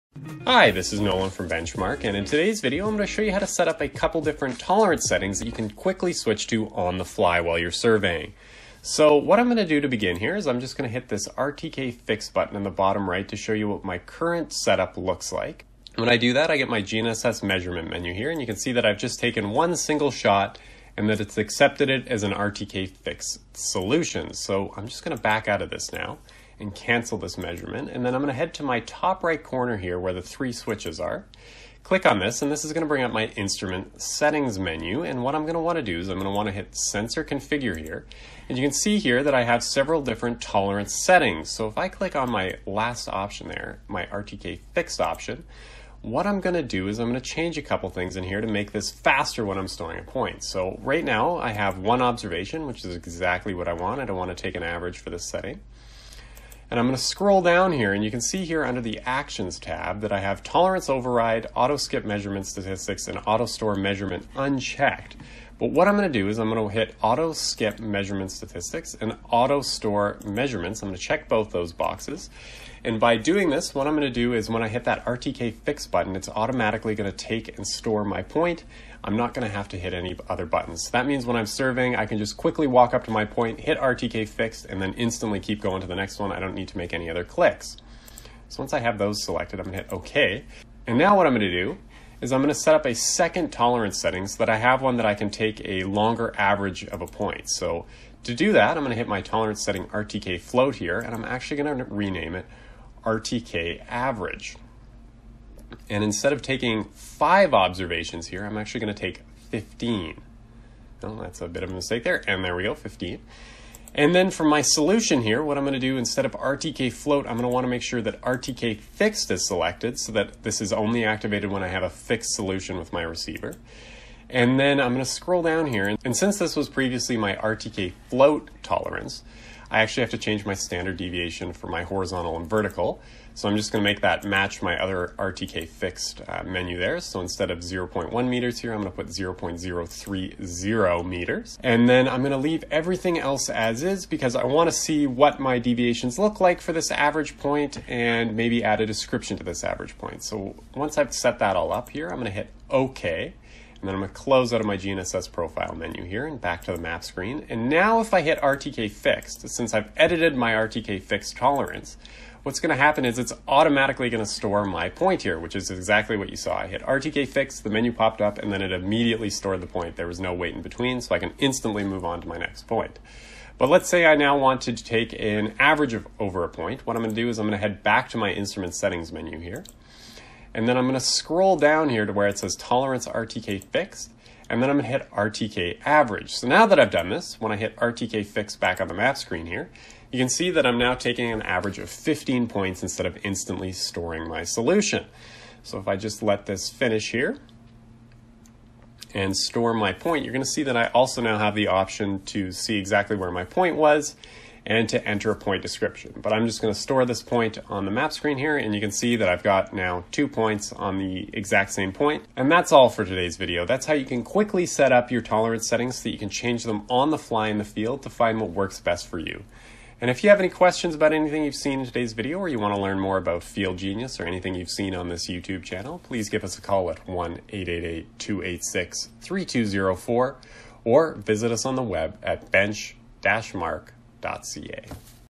MicroSurvey FieldGenius How To: QuickSet Fix Tolerances | Bench Mark
In today's video, Nolan from Bench Mark shows you how to get the most out of FieldGenius with the quickset tolerance options. Learn how to quickly set up tolerance settings that you can switch to on the fly.